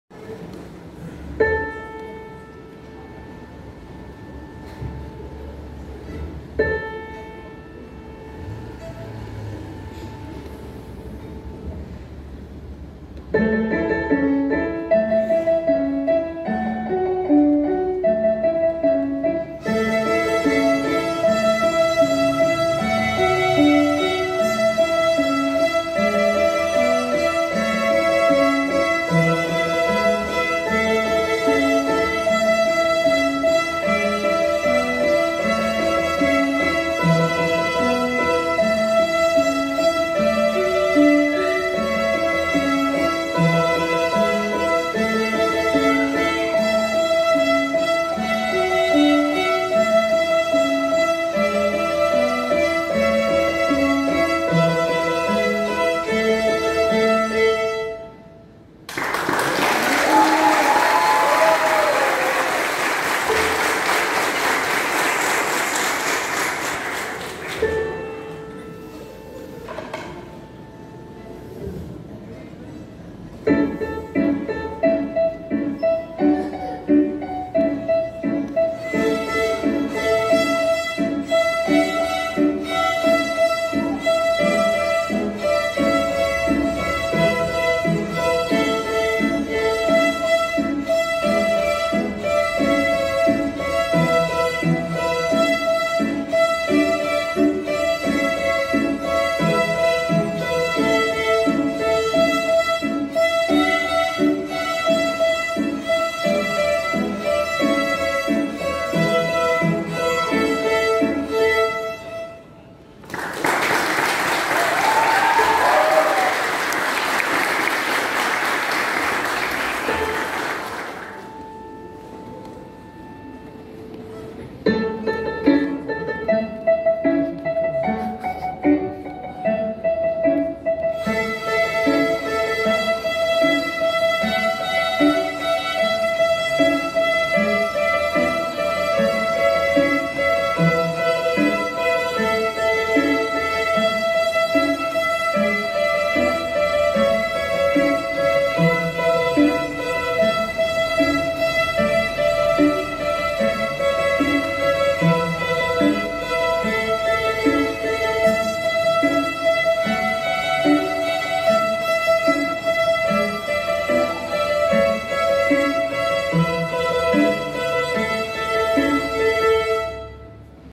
Apresentação Tunas do Paraná 2021
Projeto Cordas do Iguaçu
Prof Fernanda Boaventura
Ensino Coletivo
1a apresentação